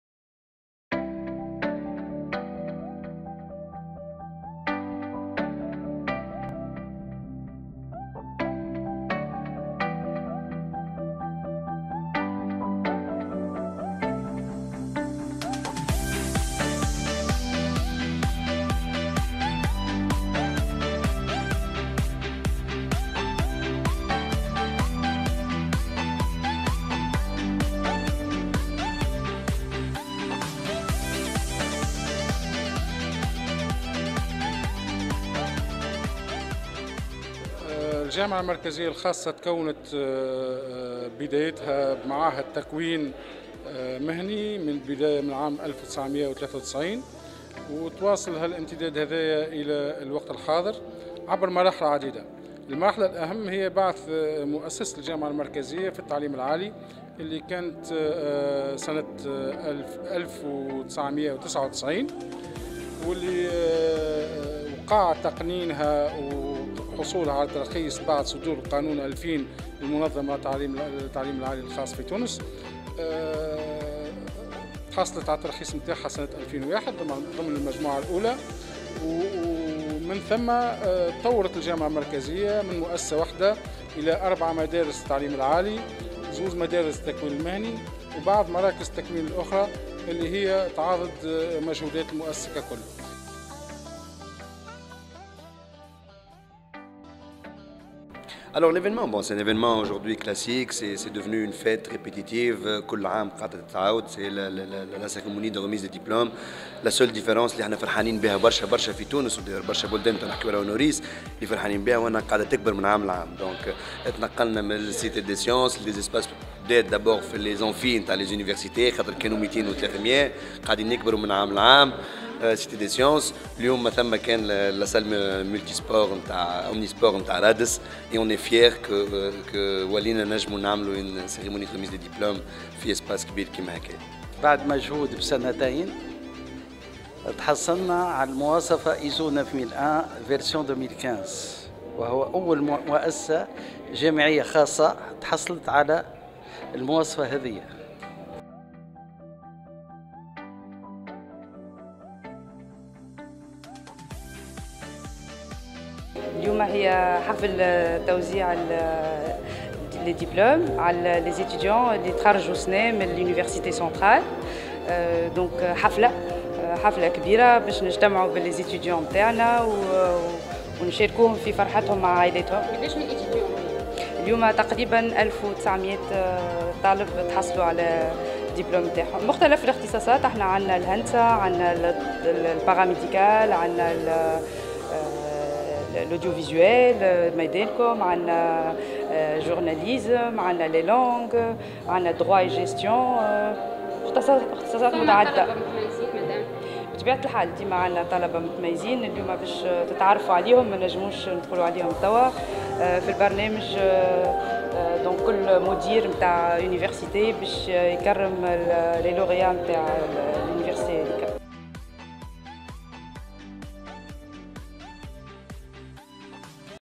La cérémonie de la remise des diplômes des étudiants de l’Université Centrale 2016/2017
L’Université Centrale de Tunis a organisé, hier, le 19 juillet 2017, sa 26ème édition de remise de diplômes à la salle multidisciplinaire du stade de Radés afin d’honorer ses étudiants